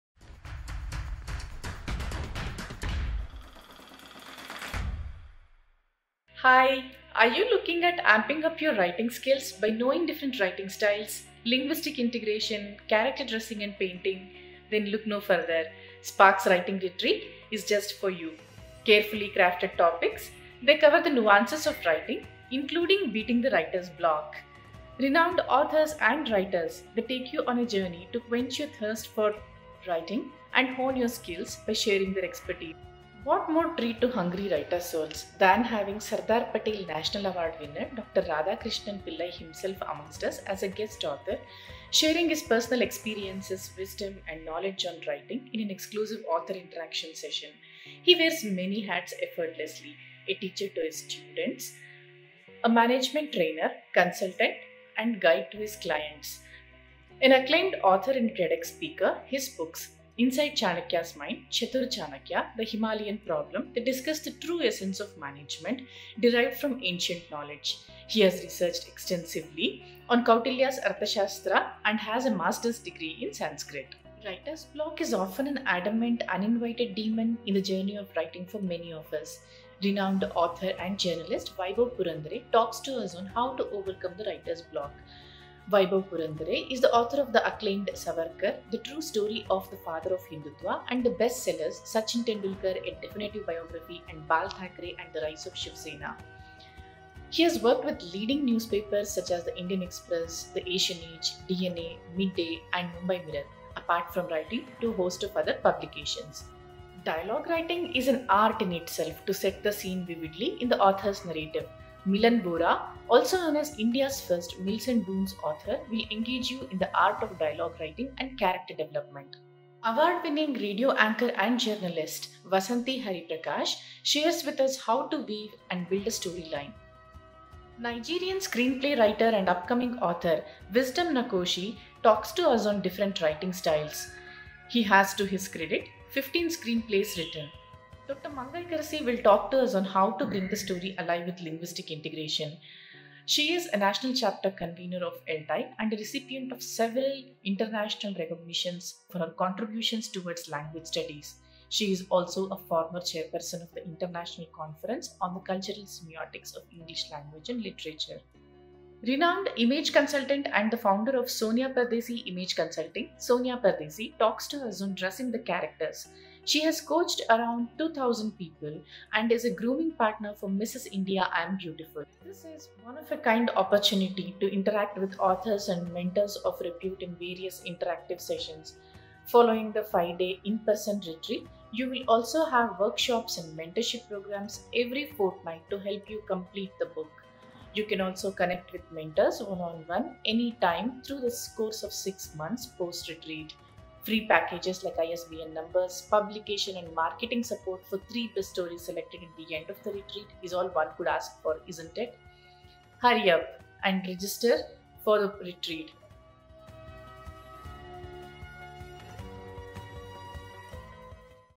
Tejaswini | Mentor | Spark Writers Retreat
About Tejaswini:

An engineering graduate by education and  a Master’s degrees in  computers science Engineering , I juggle between working in a technology company and writing. As  different backgrounds they seem so are my interests towards various forms of writing from fiction to non fiction I love them equally.
Having completed a creative writing course from “MyCaptain”  I took to writing small poems , and quotes to quench my thirst for writing . While a friend introduced me to different writing platforms like Mirakee and yourquote , my aspirations to write just got stronger .
YourQuotes published my book of quotes called “ Life Gravitas “ in 2020 .
Sparks Writing group was just the platform that was needed to raise my bar of writing .  Various writing activities  under the  umbrella of captain Sahana  Sundar along with host of other  elite writers in the group gives myriad opportunities to publish from short poems to blog posts for the group